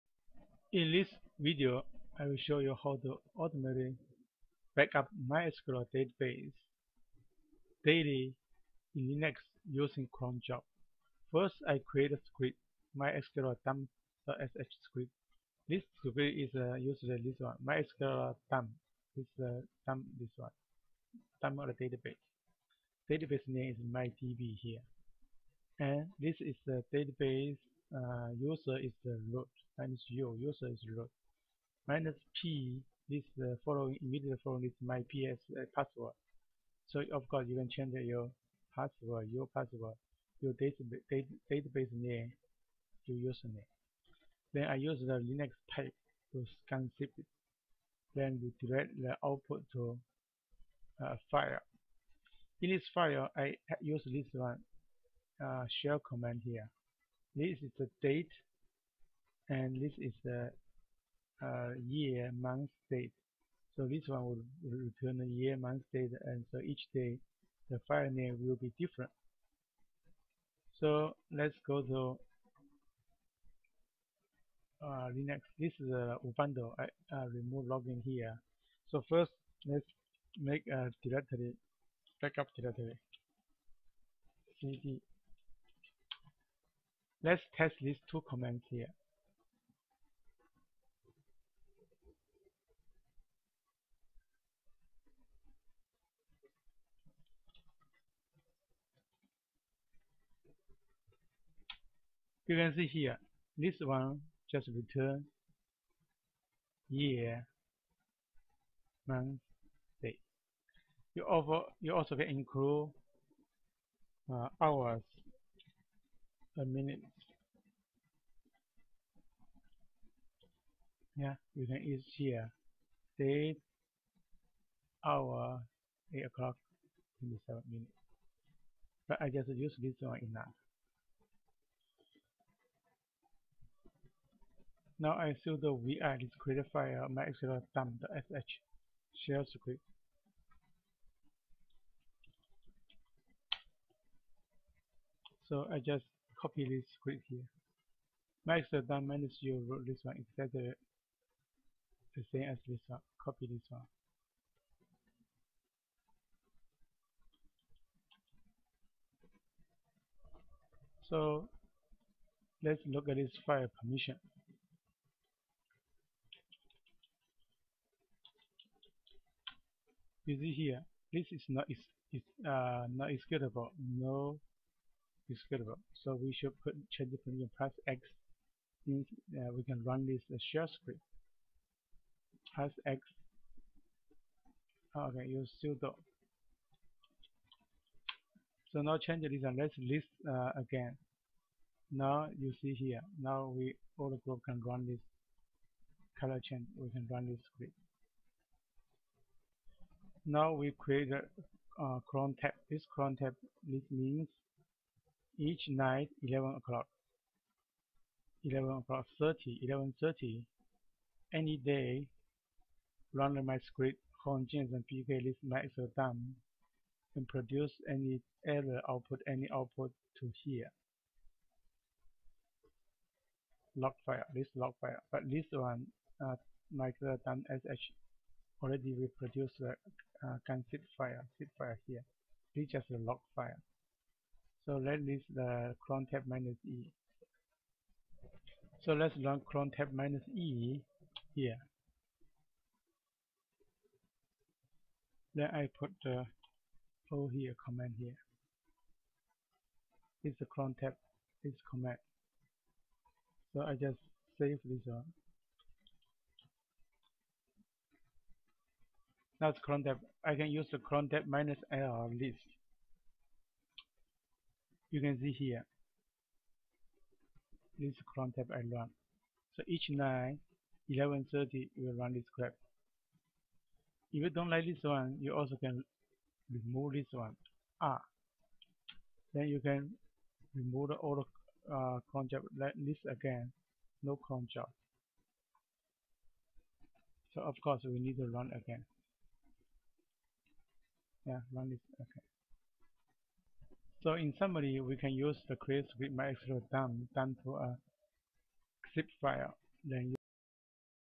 Linux, automatically backup MySQL database daily
Using cron job to backup MySQL database daily in Linux.